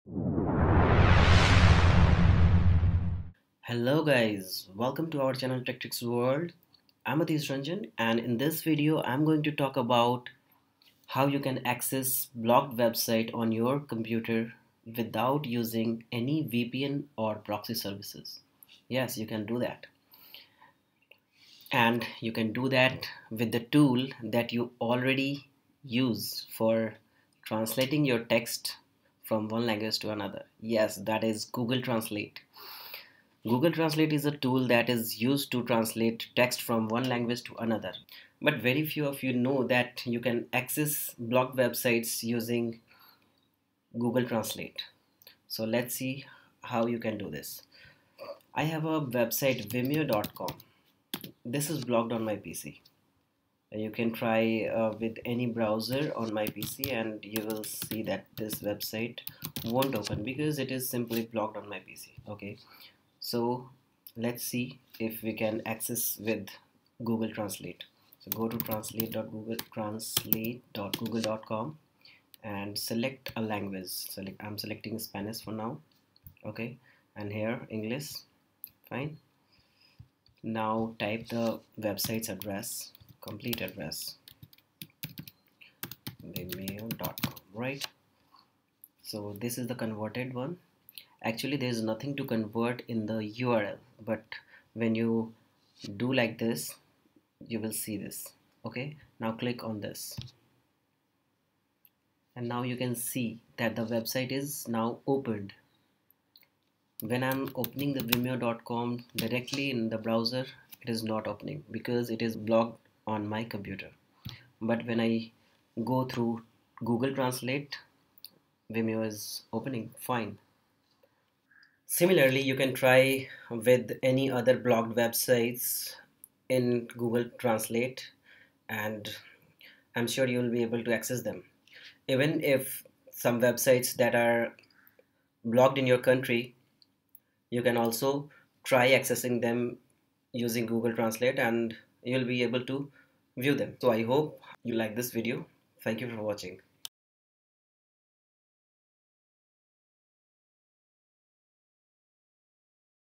How to Access Blocked Websites Without Using VPN or Proxy Services?
Accessing blocked websites may be required sometimes for some educational purposes, and you don't have any VPN or Proxy services, then this video will help you know how you can access blocked websites without the use of VPN or Proxy.